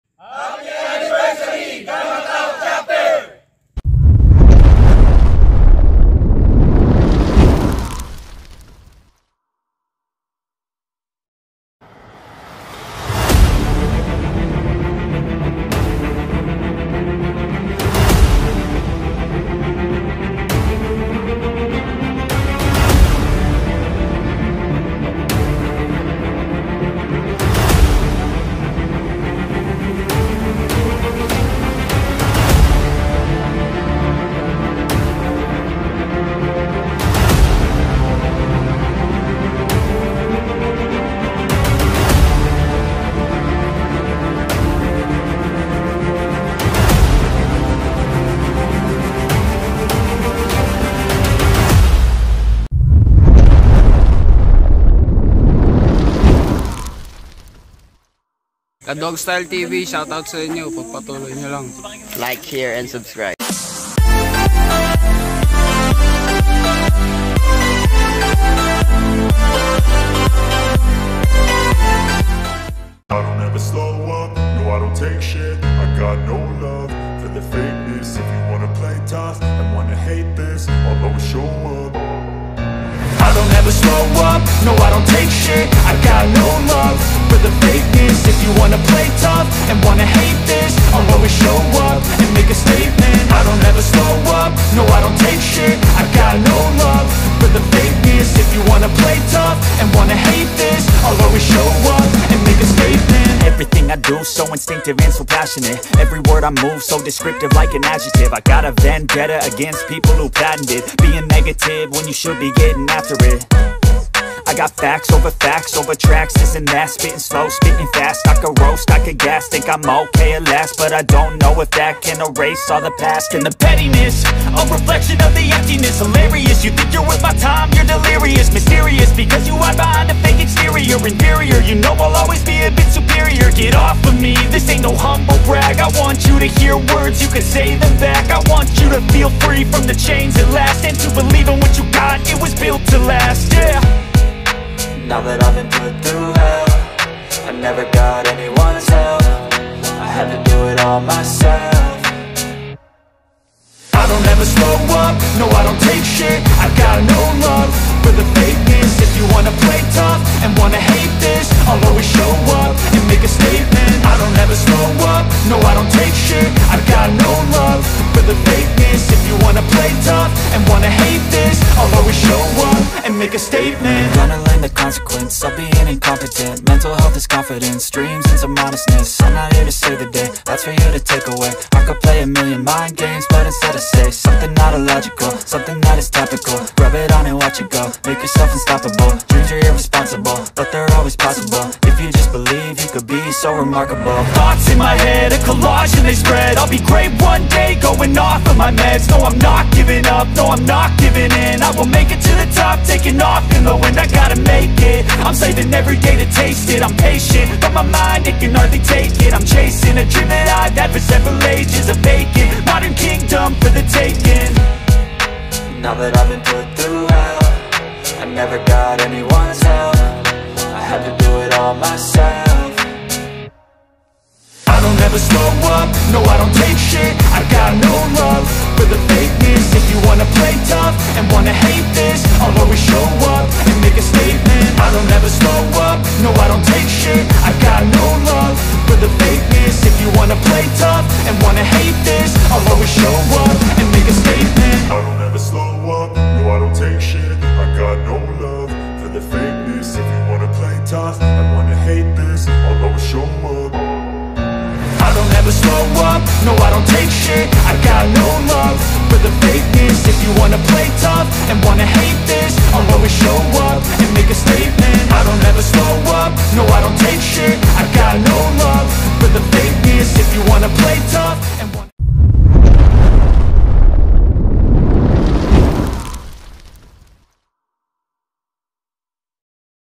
46th Founding Anniversary ALPHA PHI OMEGA (GAMMA TAU CHAPTER)
Happy 46th Founding Anniversary brothers and sisters of ALPHA PHI OMEGA (GAMMA TAU CHAPTER)

⚜️ Tree Planting
⚜️ Gift Giving
⚜️ Feeding Program

LEADERSHIP - FRIENDSHIP - SERVICE